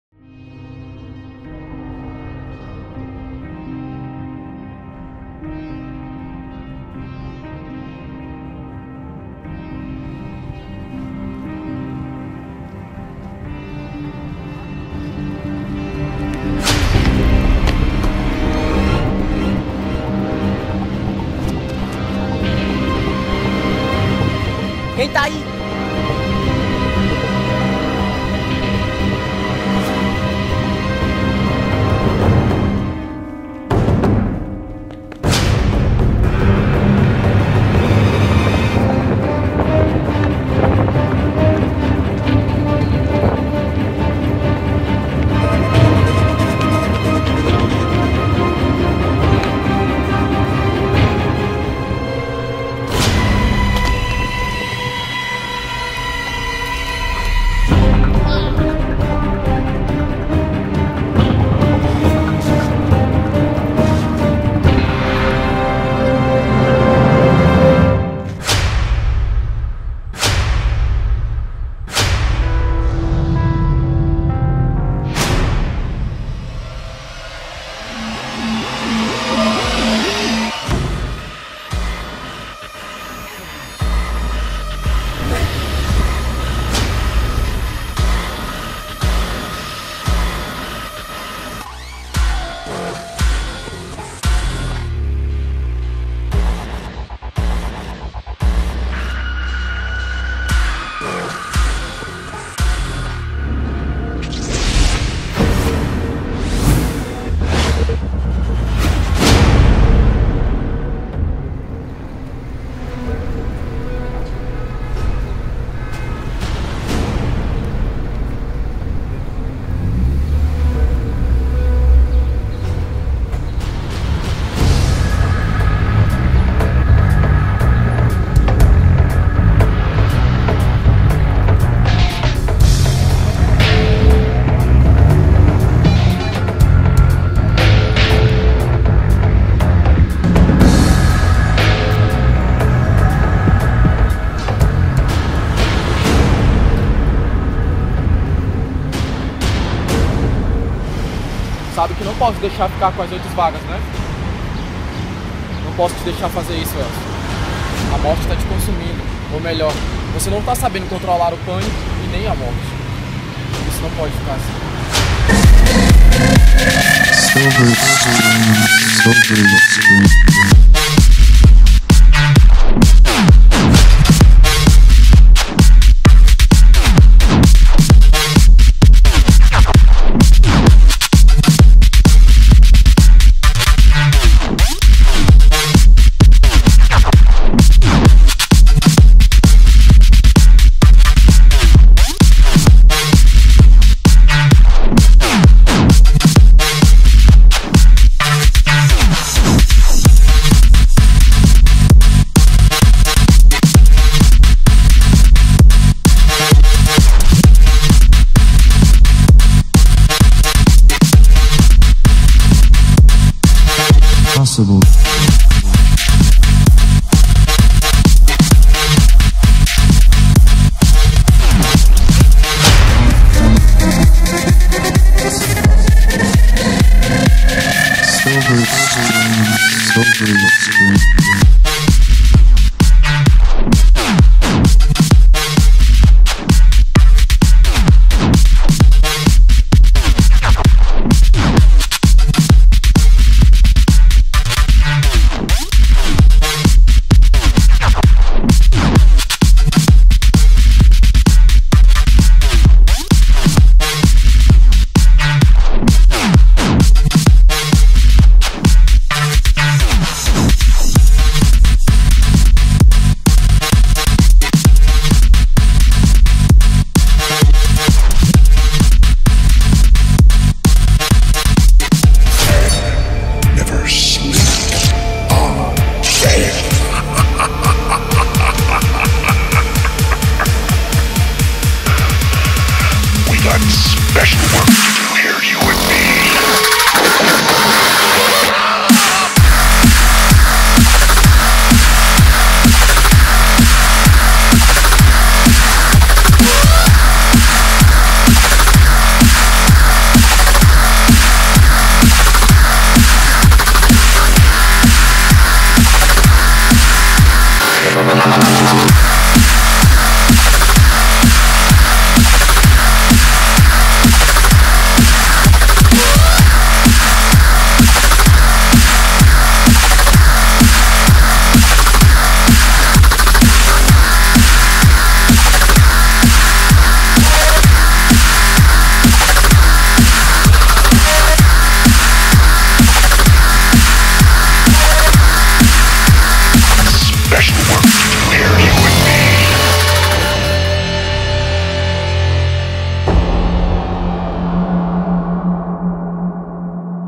DeathFight: Piauí X Maranhão - TRAILER OFFICIAL
Cidade: São Luiz do Maranhão
Local: Teatro da Cidade de São Luiz - (Antigo Cine Rox)
Dia: 31 de Outubro
Horário: 13:00 horas
Entrada: 10,00
Inscrição 20,00

SINOPSE DO TRAILER:
Com seu Trono garantido e muito poder acumulado, Heelcio Lopes é atacado por uma antiga entidade o "Panic" e agora está com interesses ambiciosos, ele anseia por todas as 8 vagas para o seu exército do Piauí.
Porém Jorge Washington também reúne seus melhores guerreiros do Maranhão para impedi-lo em um confronto épico.

Curtam nossa Page estamos sempre ativos!

Death Fight Productions ©

Créditos:
Jorge Washington & Diego Silva - ROTEIRO, PRODUÇÃO E EDIÇÃO